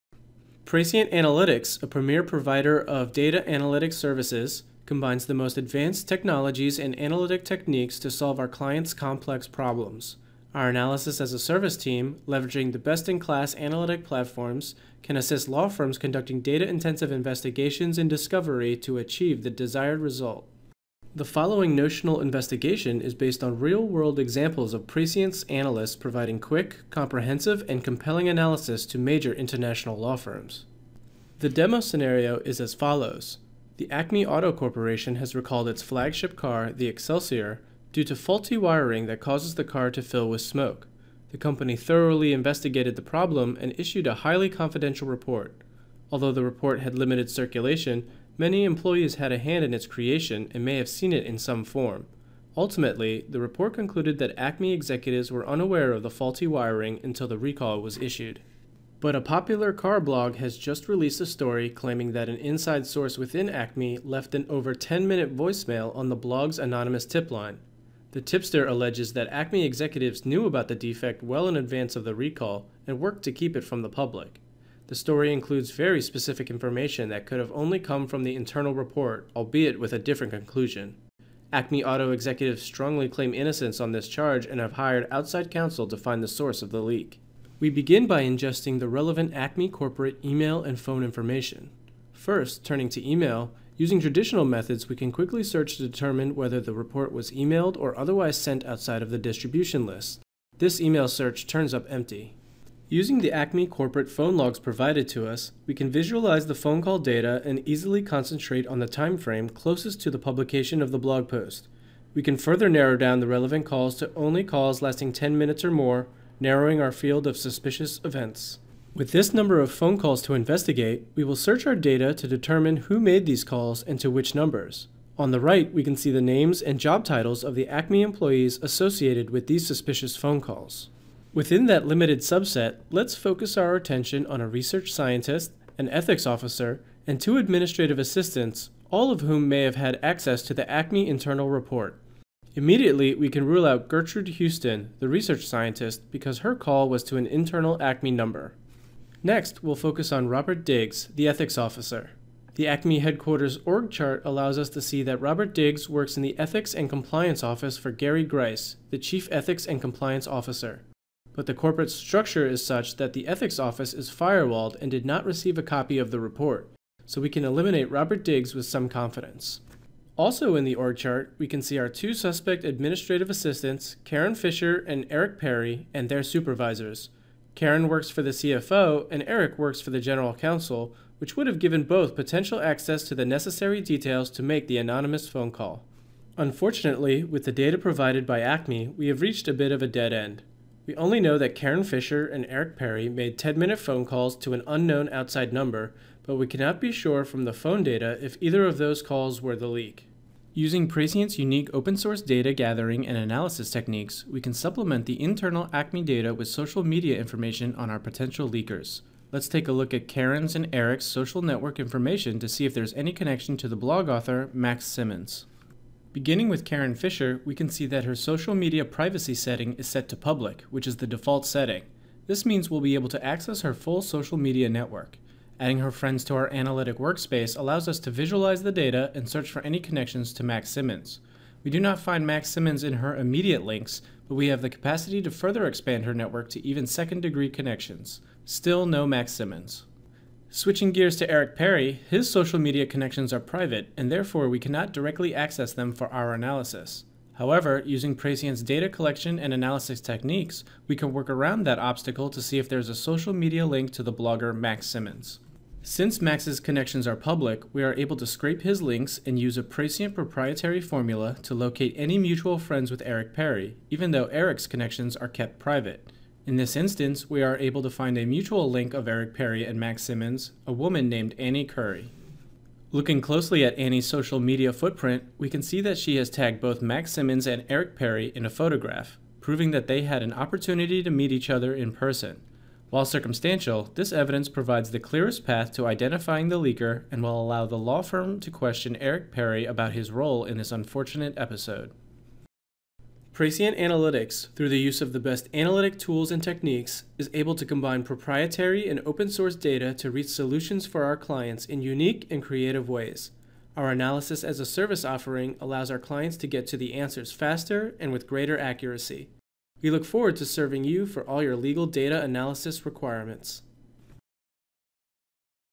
Corporate Leak Analysis
In this commercial use case demonstration, Praescient simulates a workflow that has been implemented to assist a number of international law firms. Using Palantir to dig through both proprietary and open source information, our analysts illustrate how advanced analytics can find the source of a leak of privileged information.